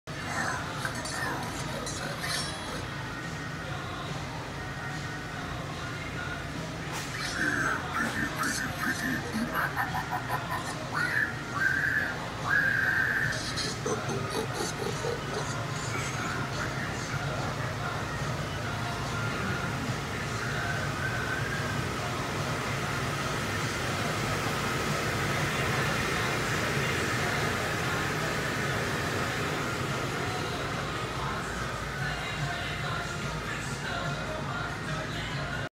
Spirit Halloween
I film this with my iPhone entering Spirit Halloween Store October 2020